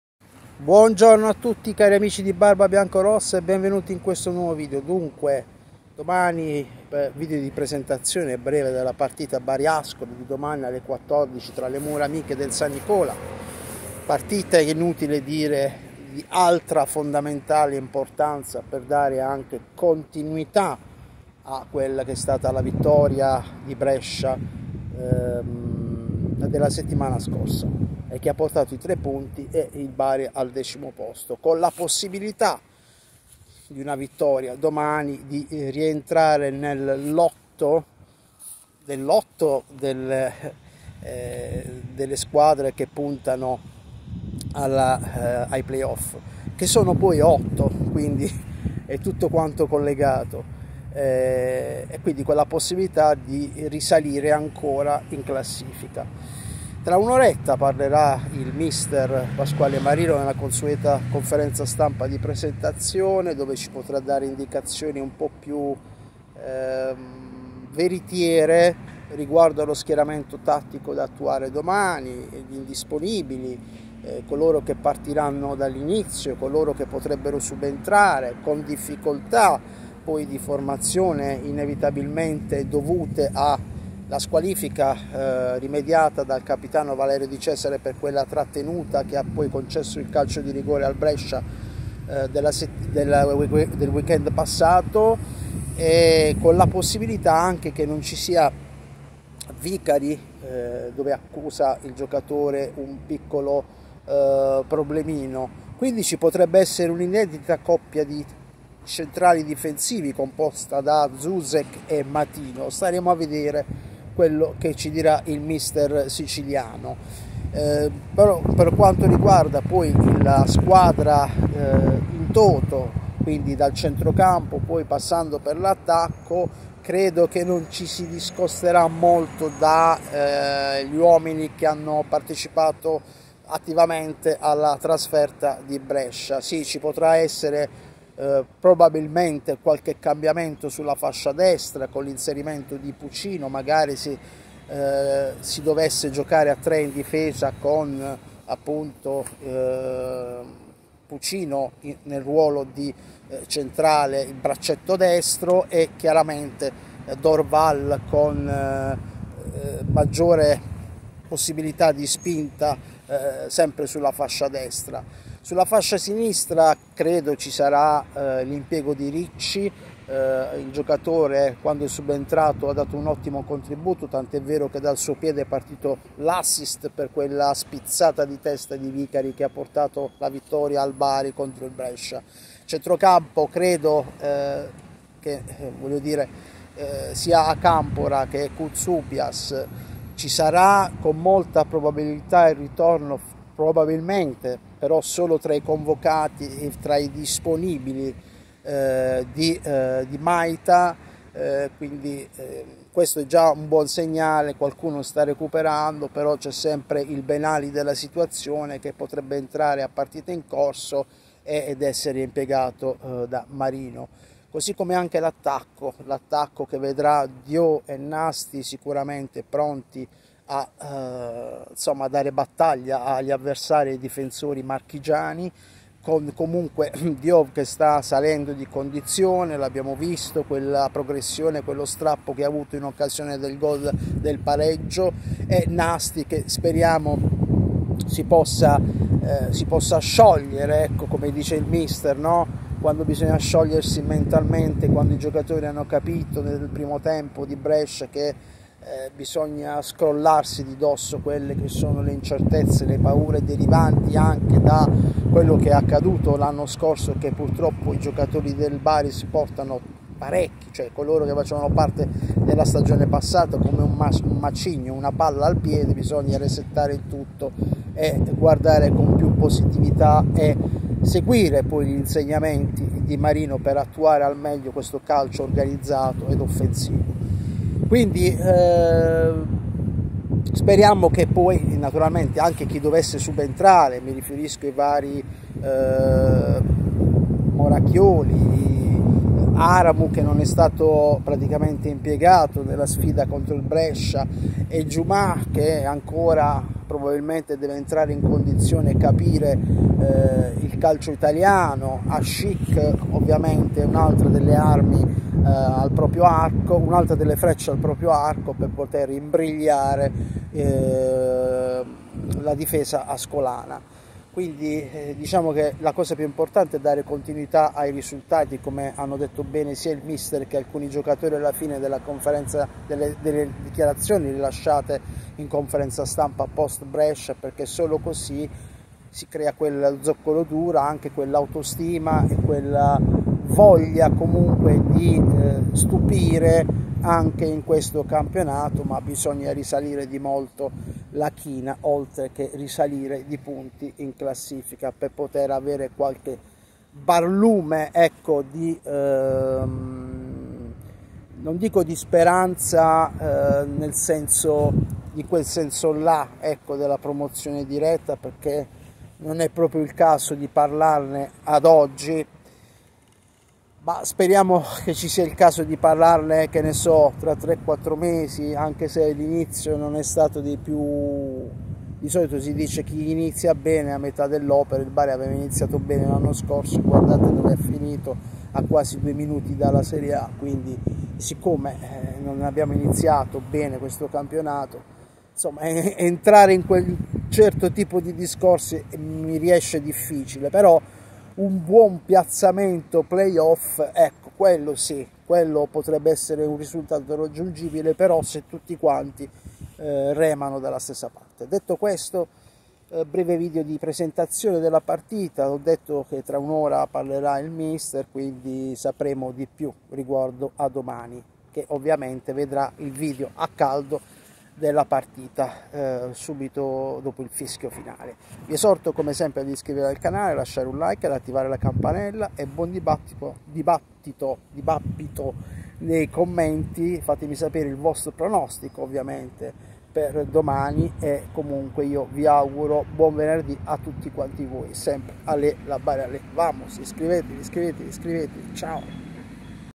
VIGILIA BARI-ASCOLI🔥||| CONTINUITÀ 💥💥
Domani ore 14:00 Bari-Ascoli allo stadio S.Nicola.
Inutile dire quanto sia importante la sfida per dare continuità alla vittoria di Brescia. In difesa qualche dubbio di formazione.